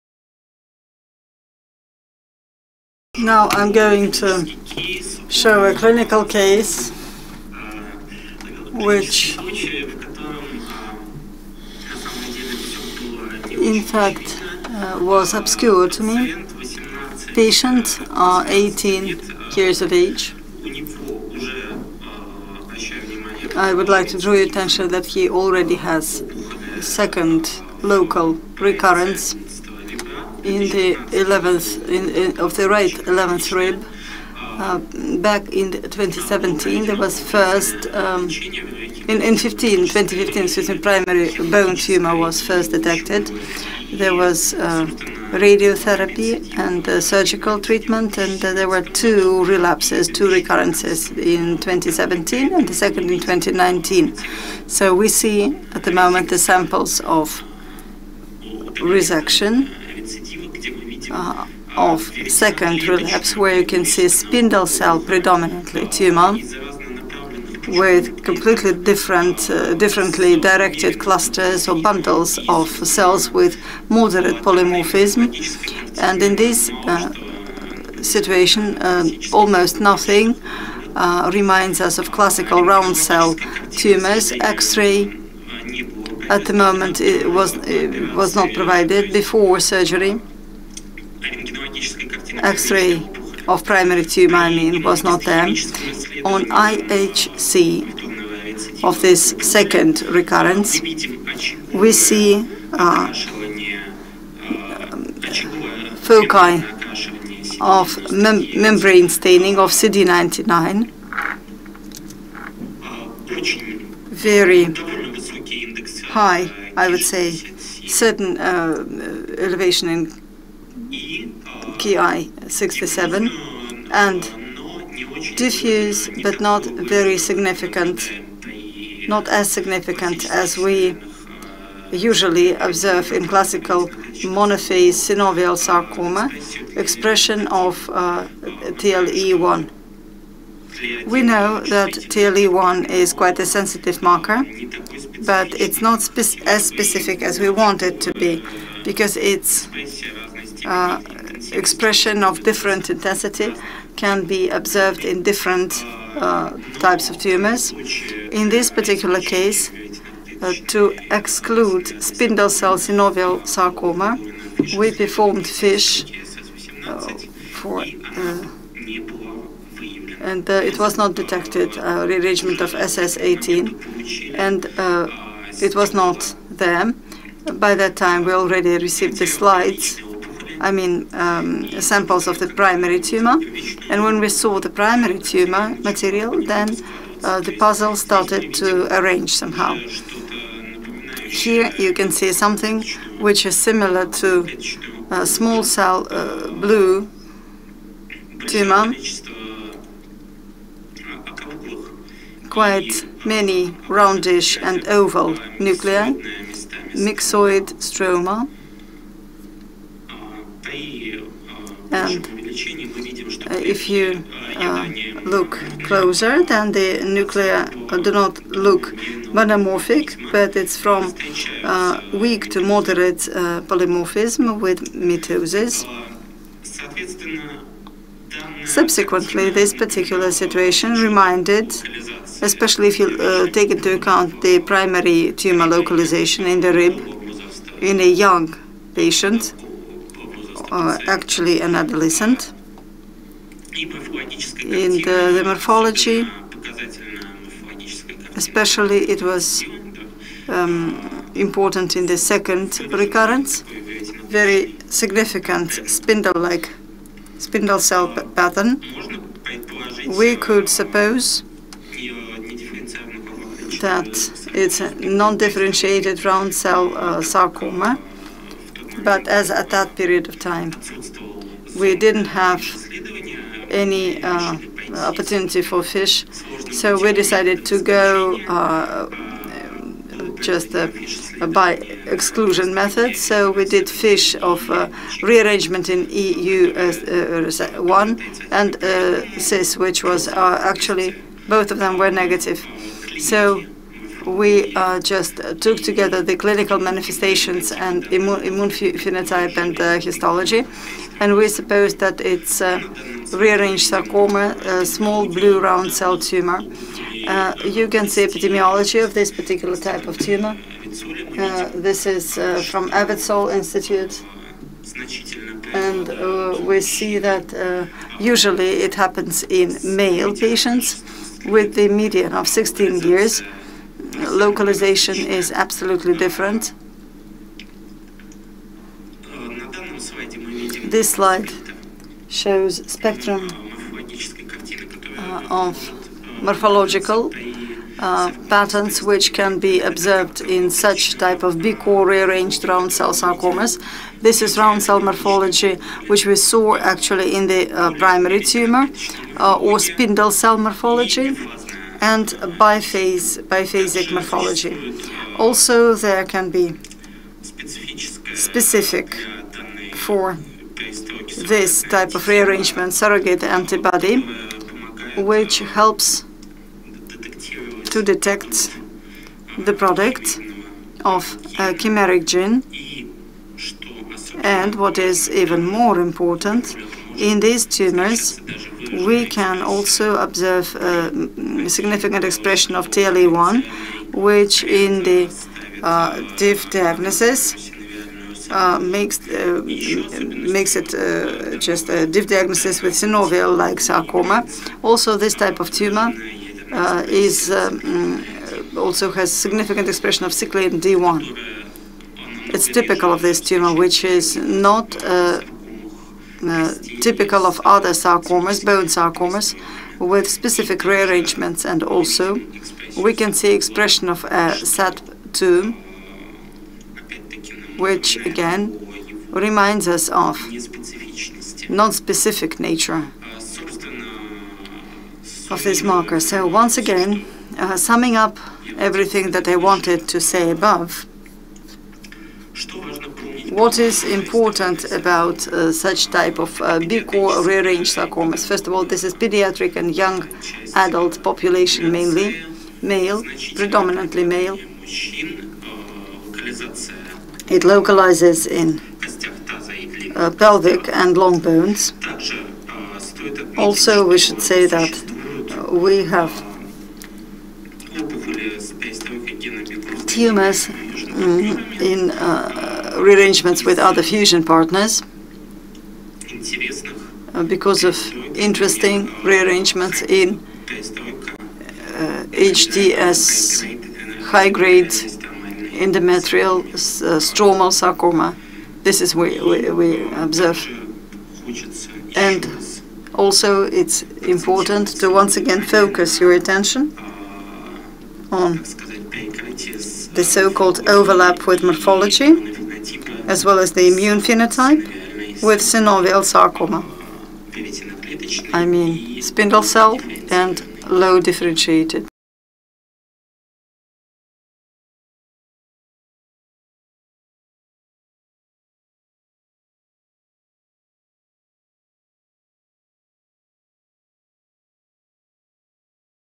Small round cell bone sarcomas with specific genetic transformations (Artem Gogolev)
Artem Gogolev - NMRC of Oncology named after N.N. Petrov of MoH of Russia, St. Petersburg

VII St. Petersburg International Oncology Forum «White Nights 2021»